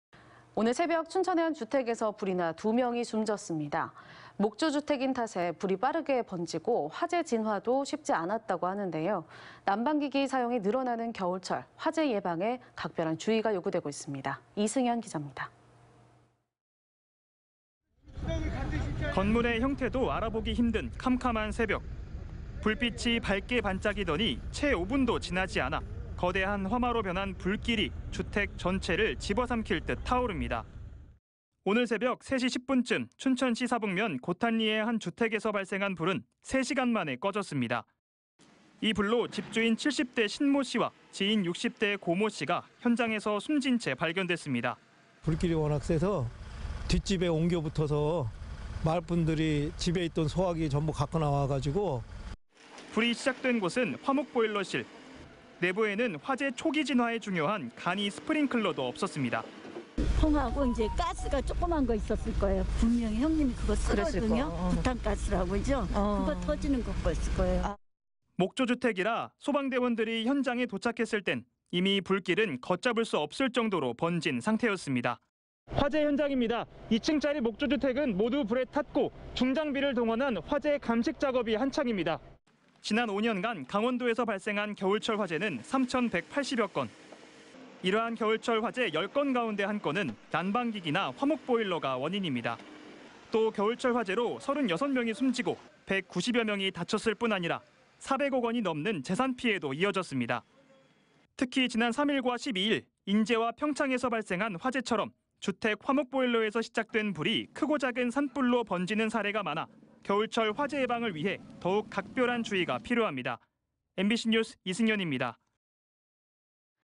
[춘천 MBC] 춘천서 화재로 2명 숨져.. 목조 주택 탓에 진화 난항 (이승연 기자)
◀ＡＮＣ▶
오늘 새벽 춘천의 한 주택에서
불이 나 두 명이 숨졌습니다.

목조 주택인 탓에 불이 빠르게 번지고,
화재 진화도 쉽지 않았다고 하는데요.

난방기기 사용이 늘어나는 겨울철,
화재 예방에 각별한 주의가
요구되고 있습니다.

이승연 기자입니다.

◀ＶＣＲ▶

건물의 형태도
알아보기 힘든 캄캄한 새벽.

불빛이 밝게 반짝이더니
채 오분도 지나지 않아
거대한 화마로 변한 불길이
주택 전체를 집어삼킬 듯 타오릅니다.

오늘 새벽 3시 10분쯤
춘천시 사북면 고탄리의 한 주택에서
발생한 불은 3시간 만에 꺼졌습니다.

이 불로 집주인 70대 신 모씨와
지인 60대 고 모씨가 현장에서
숨진 채 발견됐습니다.

[유필호/춘천시 사북면 고탄리 4반장]
"불길이 워낙 세서 뒷집에 옮겨붙어서
마을 분들이 집에 있던 소화기 전부 가지고 나와서..

불이 시작된 곳은 화목보일러실.

내부에는 화재 초기 진화에 중요한
간이 스프링클러도 없었습니다.

[인근 주민]
"펑 하고. 가스가 조그만 게 있었을 거예요.
형님이 그거 쓰거든요. 부탄가스라고 그러죠.
그거 터지는 소리였을 거예요"

목조주택이라 소방대원들이
현장에 도착했을 땐
이미 불길은 걷잡을수 없을 정도로
번진 상태입니다.

[브릿지]
"화재 현장입니다. 2층짜리 목조 주택은
모두 불에 타 흔적만 남았고,
중장비를 동원한 감식이 한창입니다.

지난 5년간 강원도에서 발생한
겨울철 화재는 3천 180여건,

이러한 겨울철 화재 10건 가운데 1건은
난방기기나 화목보일러가 원인입니다.

또, 겨울철 화재로 인한 36명이 숨지고
190여 명이 다쳤을 뿐 아니라
400억원이 넘는 재산피해도
이어졌습니다.

특히, 지난 3일과 12일
인제와 평창에서 발생한 화재처럼
주택 화목보일러에서 시작된 불이
크고 작은 산불로 번지는 사례가 많아
겨울철 화재 예방을 위해
더욱 각별한 주의가 필요합니다.

mbc뉴스 이승연입니다.(영상취재:김유완)